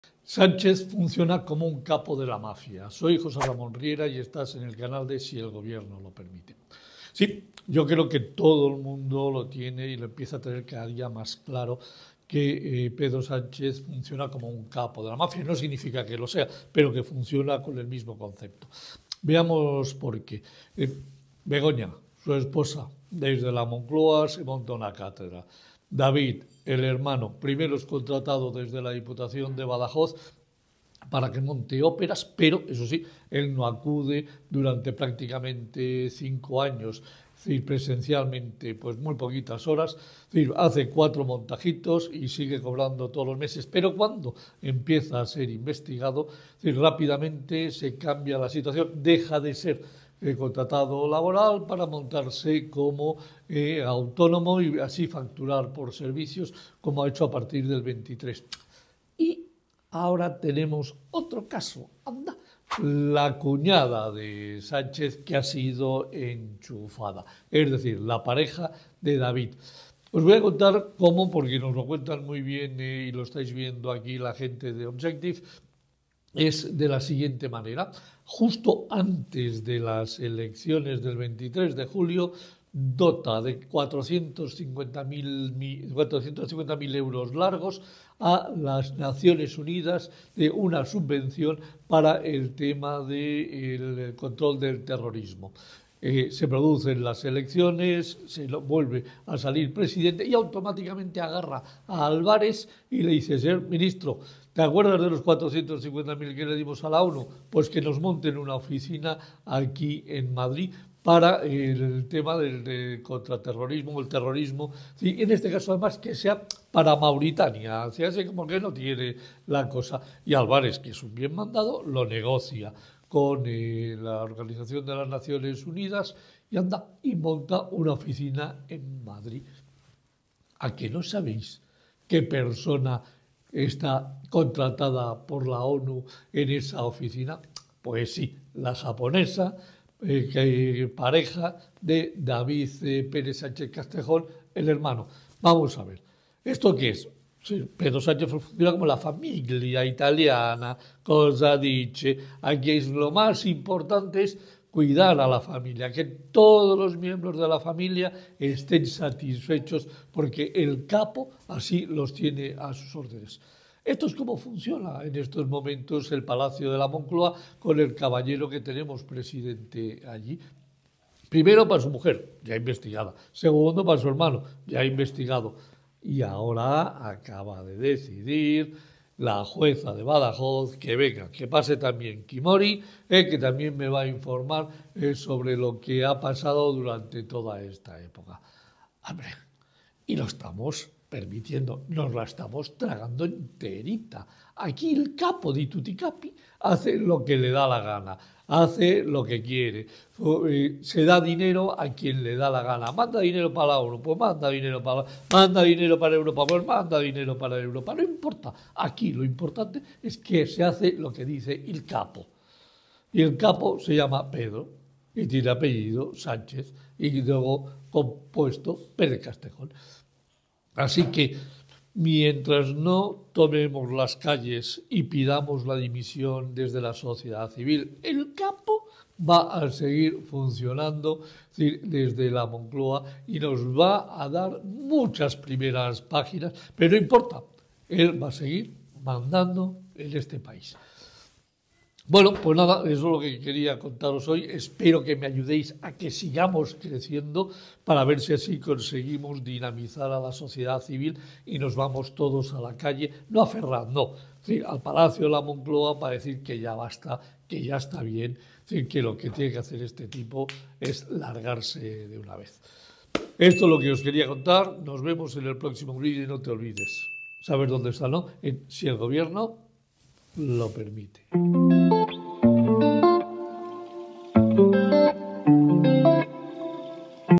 Sánchez es como un capo de la mafia
Recientemente nos hemos enterado de que la pareja del hermano de Sánchez, David, una japonesa llamada Kaori Matsumoto, ha sido contratada para un puesto de la ONU,  en la Oficina de las Naciones Unidas contra el Terrorismo (UNOCT) para Mauritania que se va a gestionar desde Madrid, oficina financiada con fondos españoles... Me explicarán ustedes si esto no son prácticas mafiosas al más puro estilo de la "Cosa Nostra". Gracias a @TheObjective_es por esta información